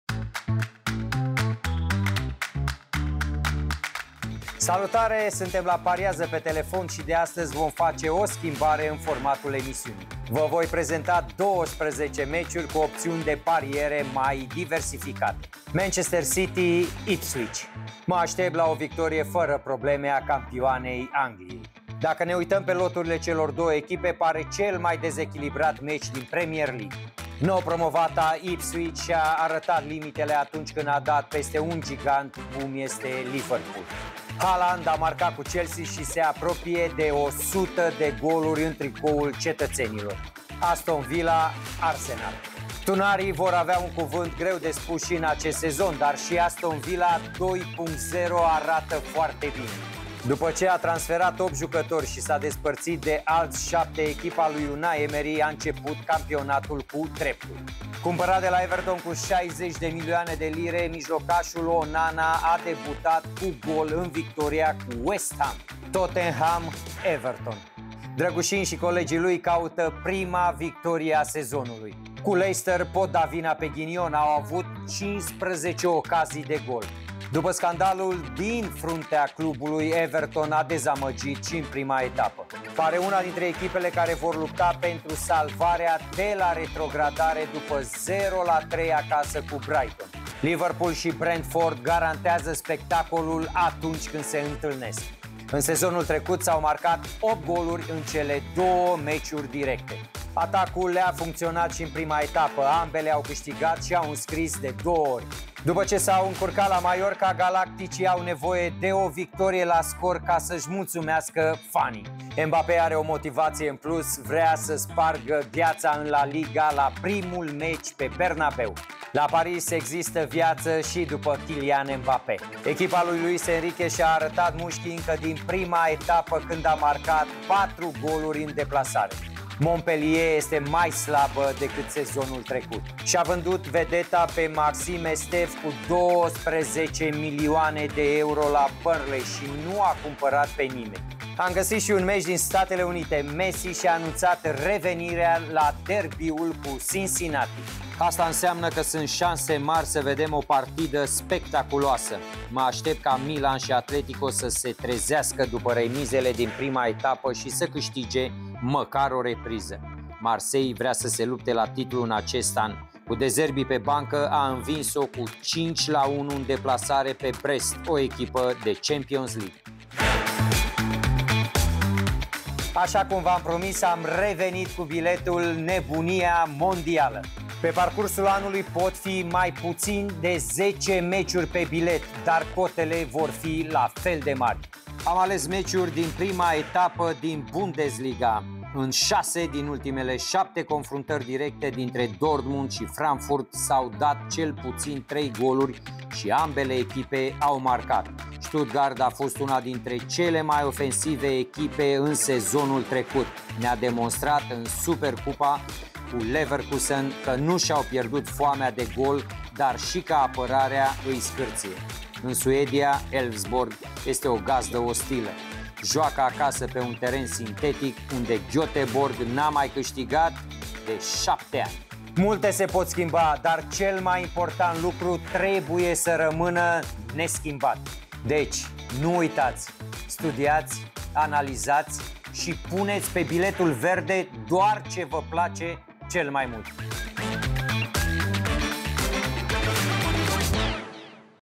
Meciurile săptămânii la ”Pariază pe telefon”. 12 meciuri alese de Pandi
Pandi a ales 12 partide pentru această săptămână la rubriaca ”Pariază pe telefon”

Analizați-le și alegeți doar ce vi se pare mai interesant.

”Pariază pe telefon” este susținut de Superbet